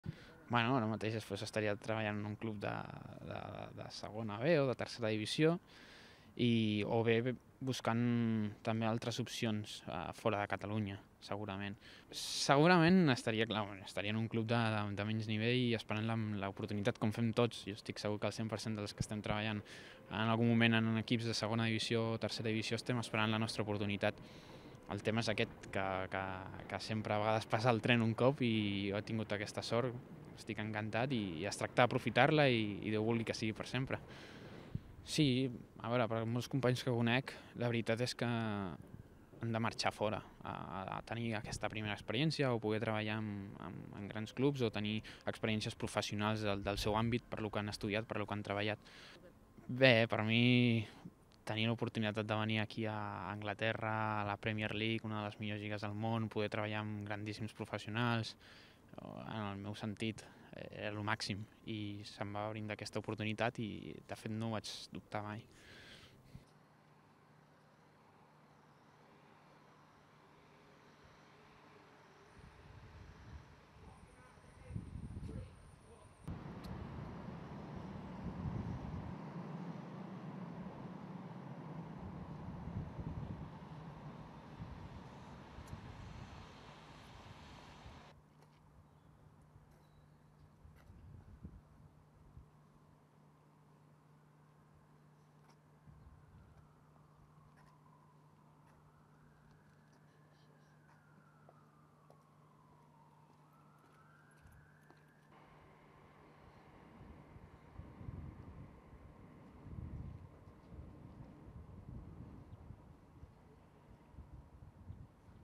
Interview with Cesc Fàbregas' personal trainer, Catalan Juan Ferrando
Catalan Juan Ferrando is the personal trainer of the Arsenal's captain, Cesc Fàbregas. Ferrando is training up some of the best football players in the English league. He left Catalonia looking for greater working experiences.